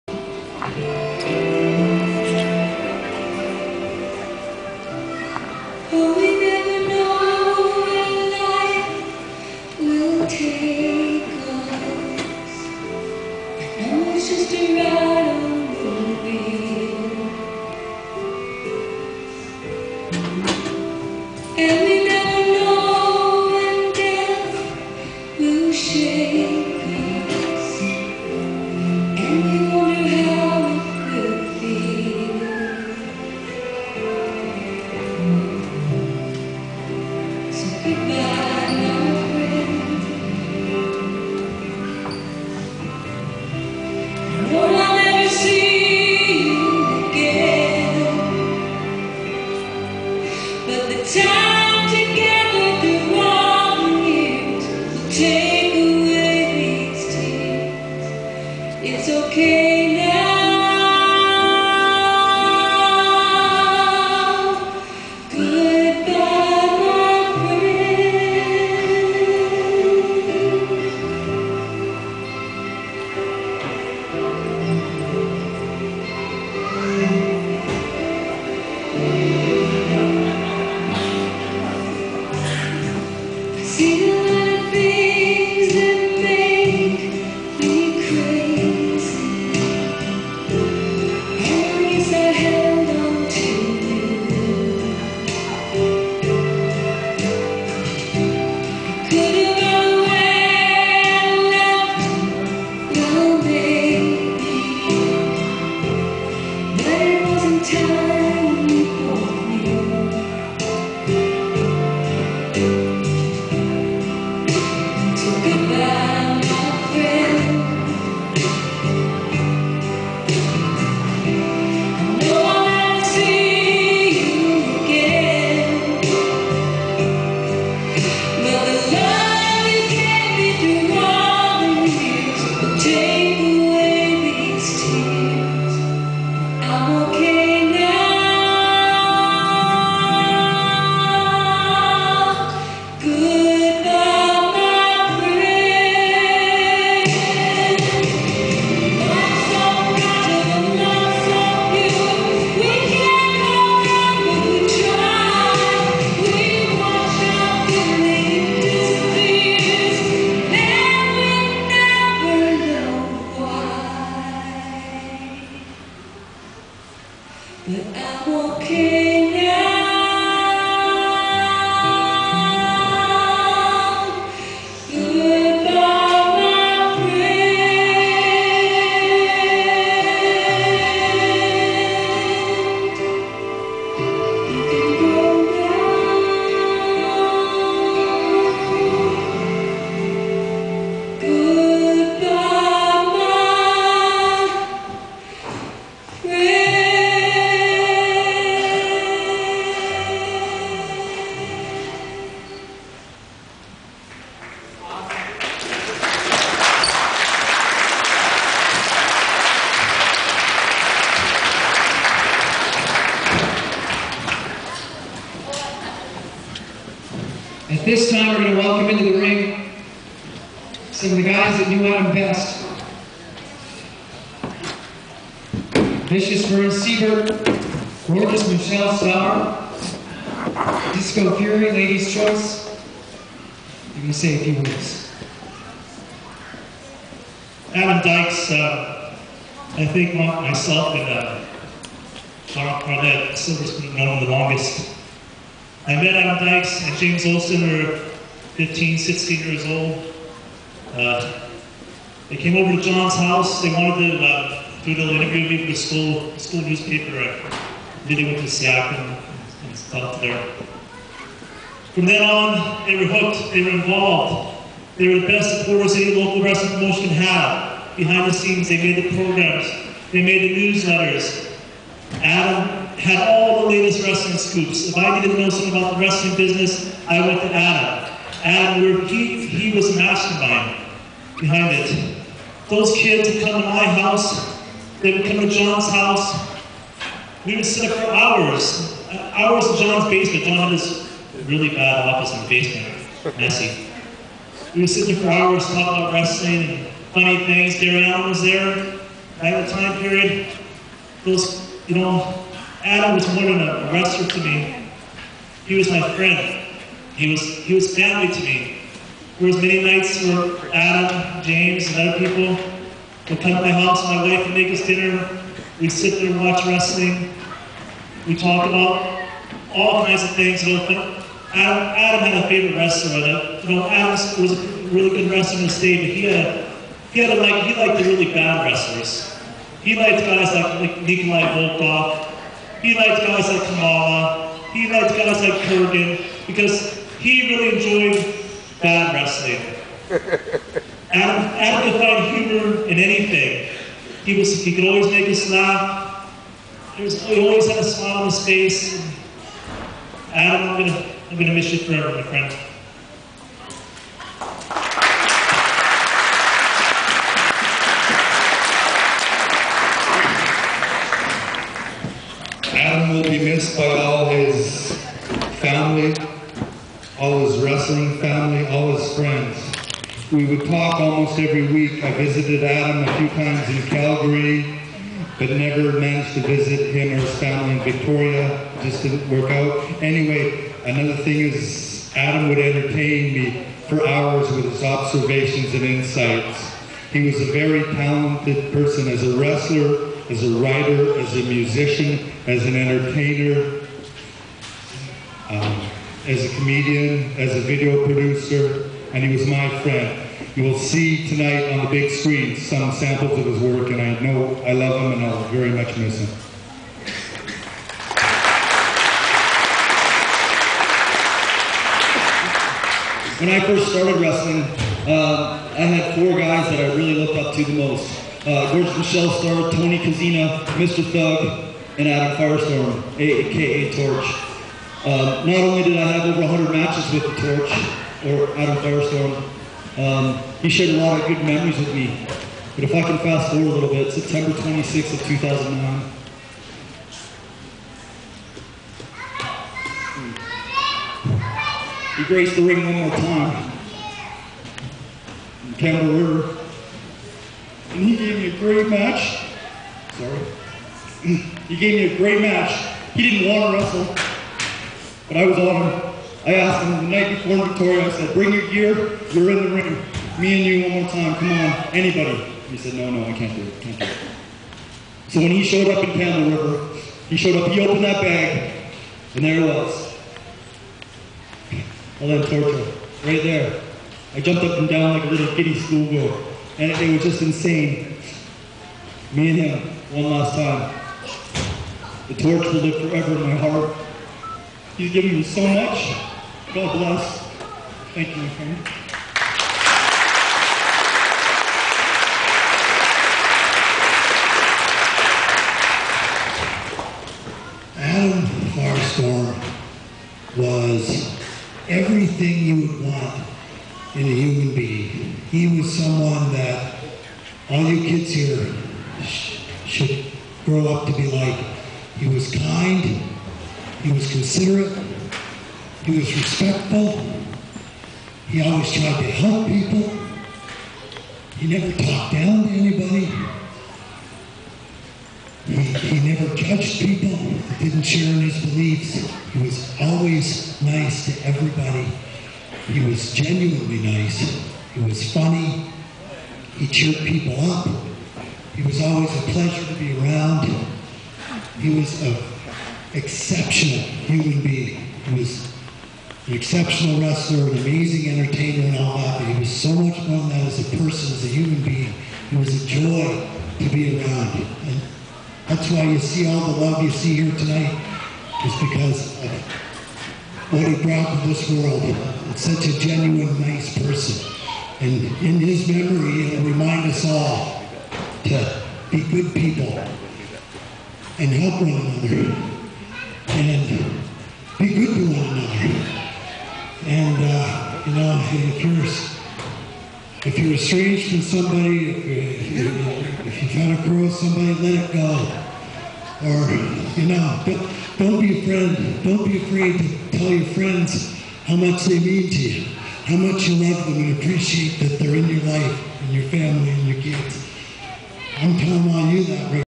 Adam Firestorm Tribute 1 of 2 19 Nov 2009
Adam Firestorm remembered with a slideshow and moving tributes from his friends and family in the Pacific Northwest Wrestling Community.